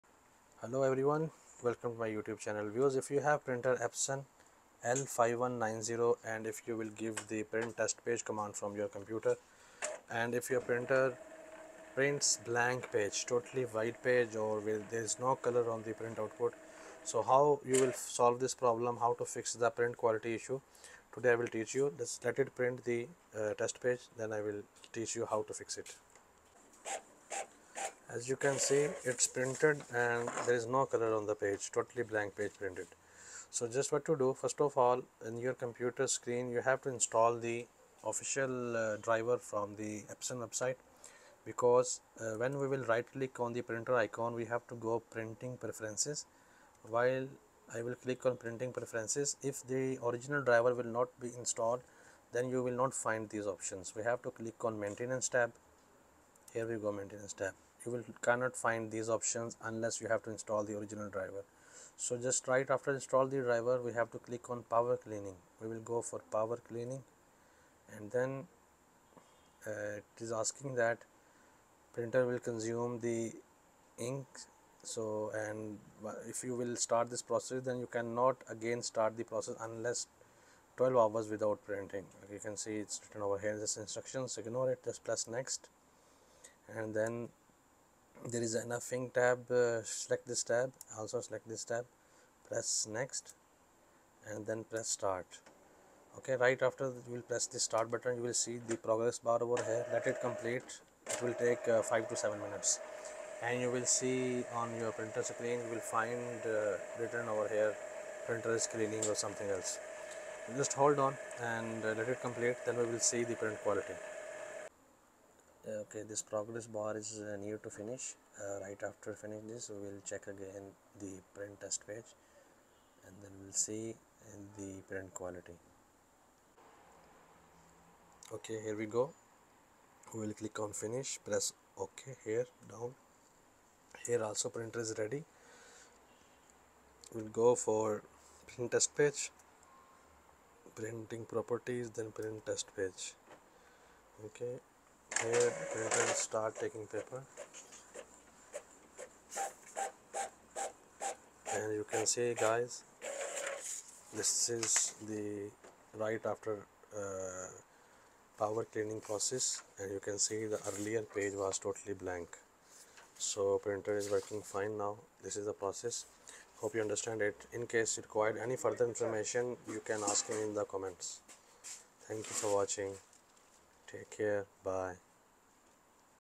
My Epson Printer is printing blank pages - What can I do to fix it?
If you're having problems with poor print quality or blank pages printing from your Epson L5190 printer, then this video is for you! In this video, we'll show you how to fix the problem and get your prints back up to standards.

Chances are, you're experiencing some problems with your Epson L5190 printer. In this video, we'll show you how to fix the common print quality issues and get your prints back up to standard. We'll guide you through the steps necessary to correct the problem and get your prints printing like new again! So don't wait any longer - watch this video and fix your poor print quality today!

Bilal's Workshop - Technologist and Vlogger